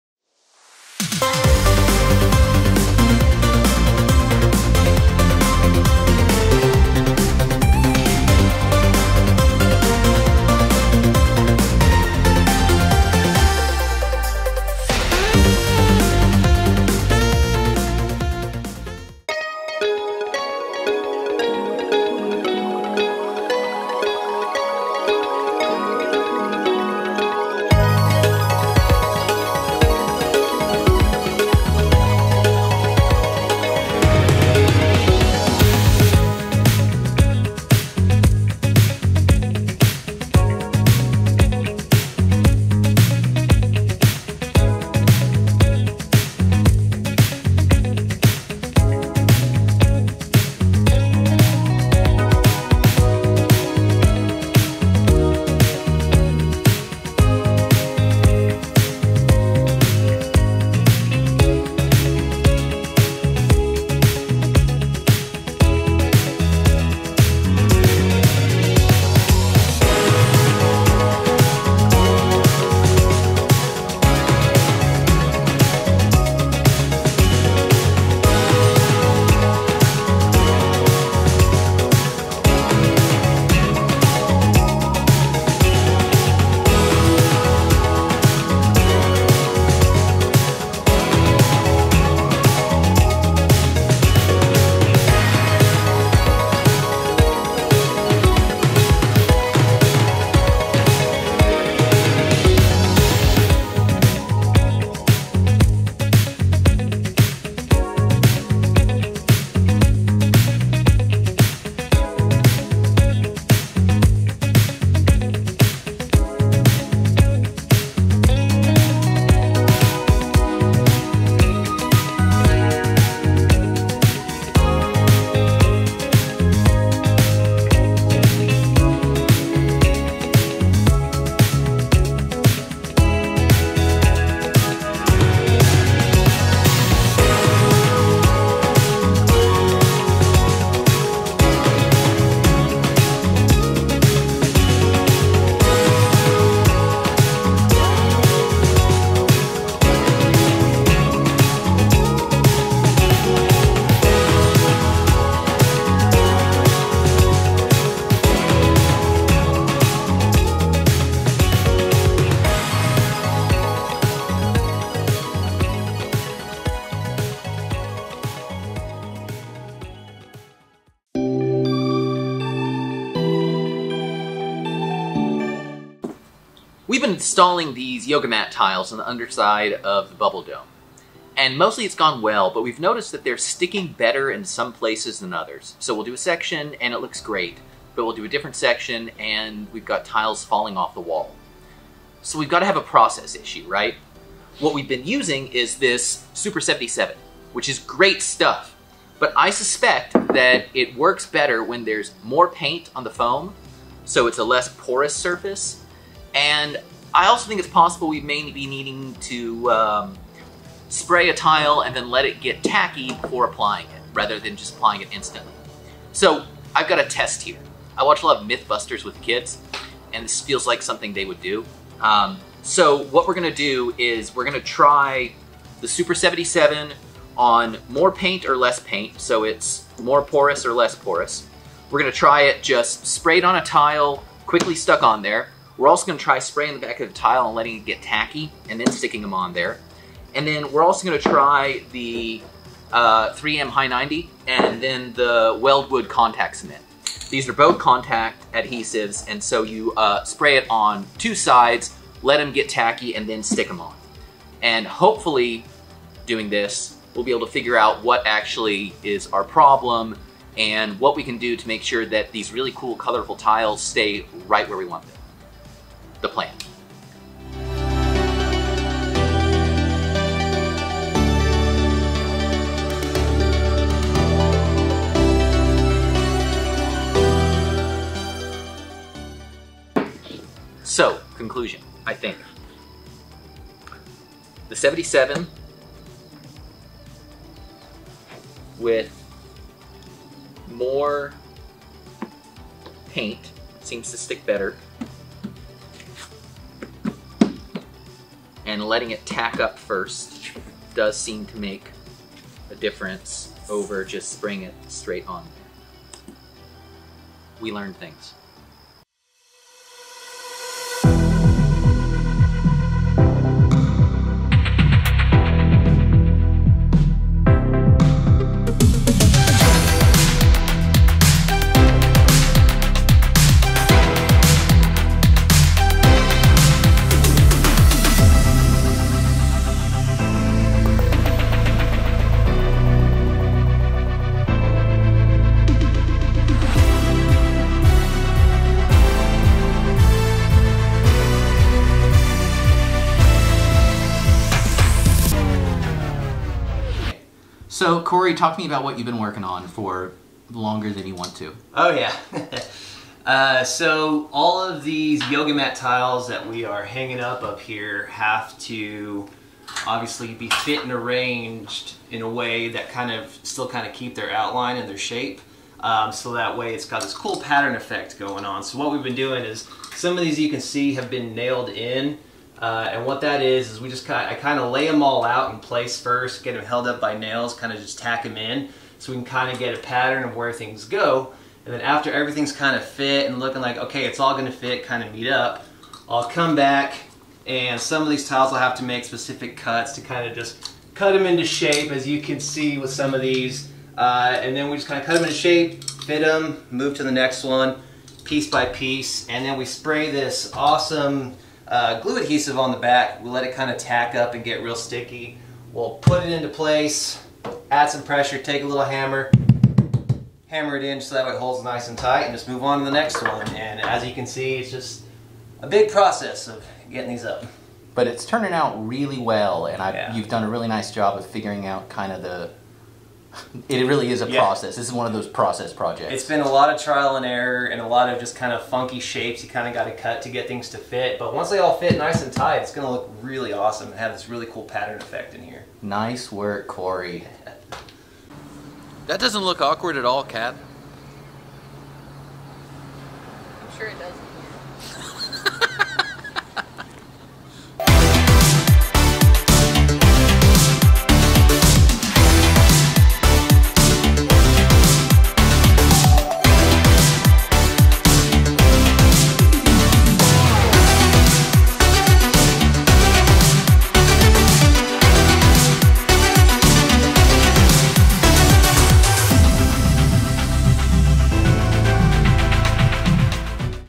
Spaceship Windows
The bubble dome that we built earlier this summer has been a very worthwhile addition to the spaceship! Tox is finished painting it and now the windows are finally installed. This week take a look at that process!

We also did some testing and troubleshooting of our yoga mat tile process. Ben made a Mythbusters inspired test of the different ways to adhere the mat tiles to the walls, and Corey really mastered it.

If you ever find yourself trying to get tiles made from yoga mats to stick to a wall, we recommend painting the foam with a couple heavy coats of latex paint, spraying 3M Super 77 onto the back of the tile, letting it tack up, and then adding plenty of pressure to the tile as it is adhered.

Also this week, Ben crashed the drone... again. At least we got some good footage of the finished spaceship front! Ben has repaired this DJI Air 2S drone before, so wish him luck this time too.